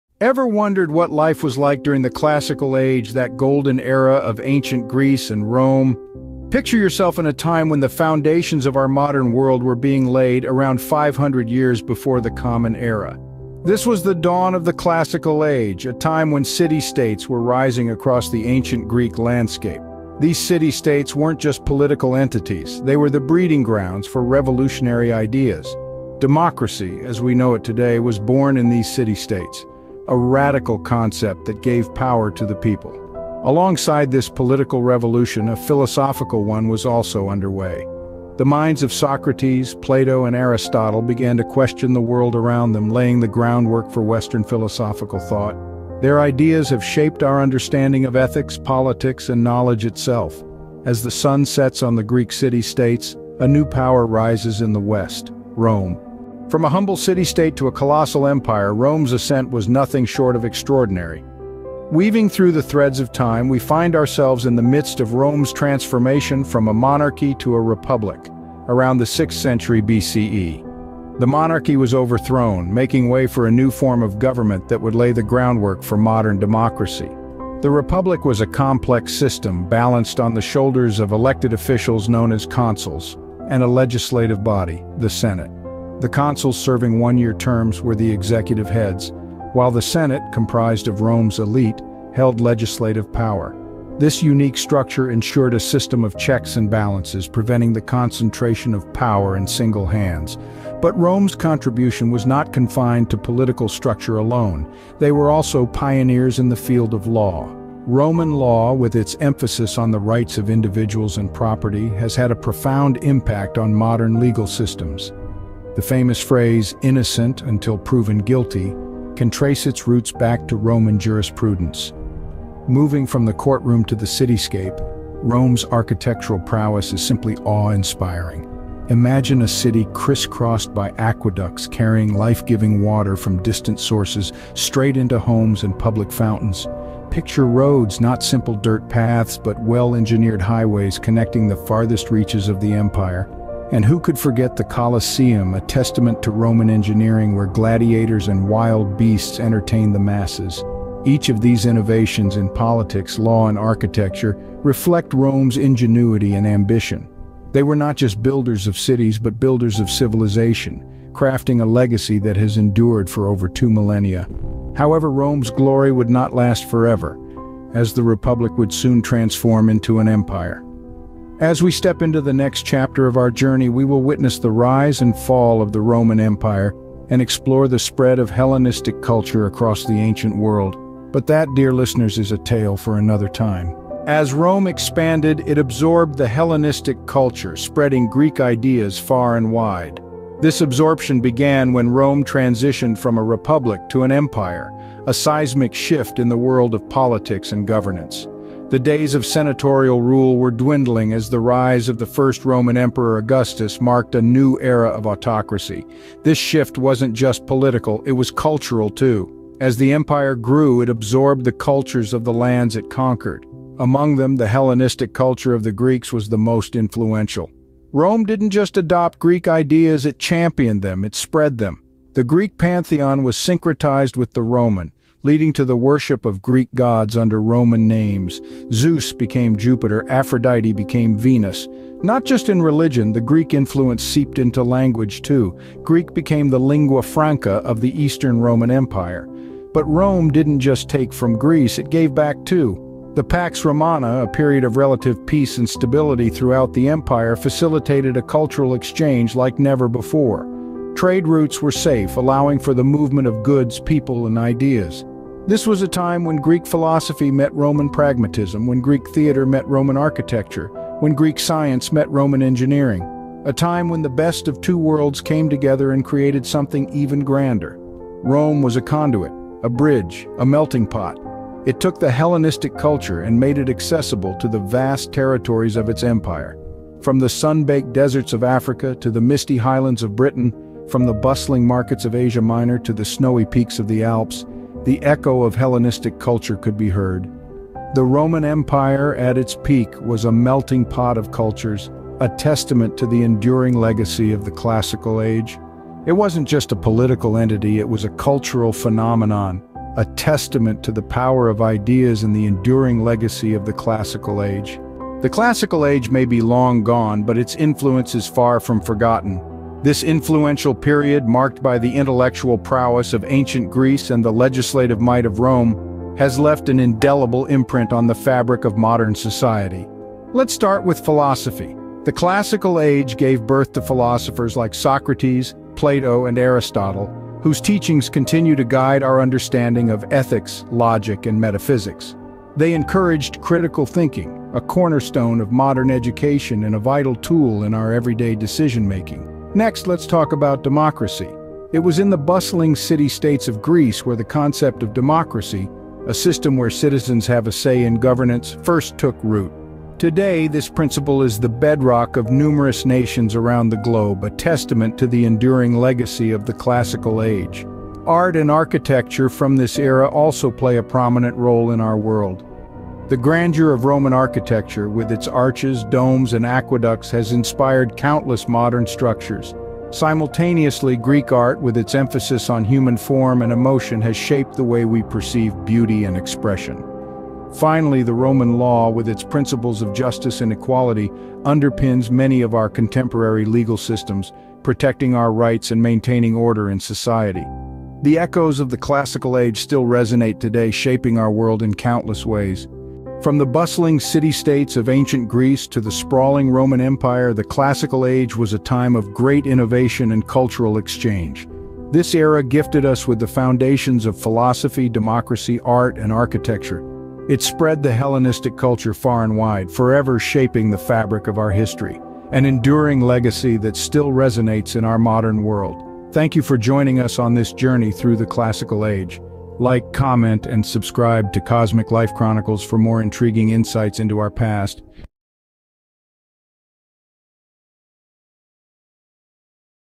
The Classical Age Unveiled: Legacy of Ancient Greece and Rome in Philosophy, Democracy, and Art 🏛️
oin us in an intellectual odyssey through the "Classical Age" as we delve into the profound contributions of ancient Greece and Rome. Explore the birthplaces of philosophy, witness the evolution of democracy, and marvel at the artistic and architectural masterpieces that defined an era. Uncover the far-reaching influence of Hellenistic culture and the indelible impact of the Roman Empire. Immerse yourself in the richness of history, as we unravel the enduring legacy of the Classical Age that continues to shape our world. 🌐🏺